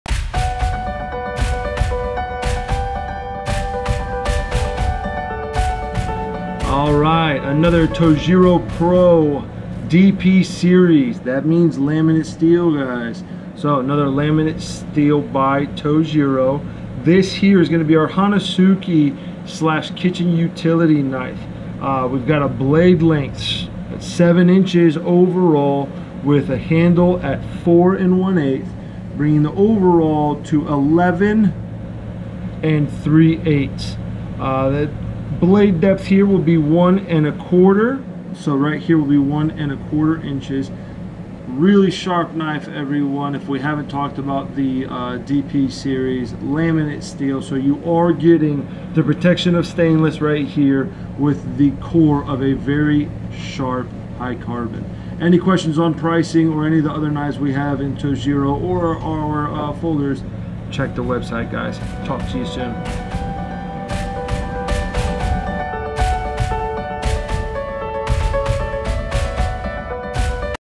TOJIRO PRO DP 3Layered by VG10 Petty Knife 180mm F845
VG10 as core steel, clad by 13-chrome stainless steel, it has achieved both professional sharpness and easy maintenance.
Handle is hygienic all stainless steel with texture so called “tornado pattern” which prevents wet or greasy hand from slipping off.

F845
Blade Length:  7”
Handle Length:  4 1/8”
Blade Depth:  1 1/4”
Handle Depth:  3/4”
Blade Thickness:  .068”
Handle Thickness:  .658”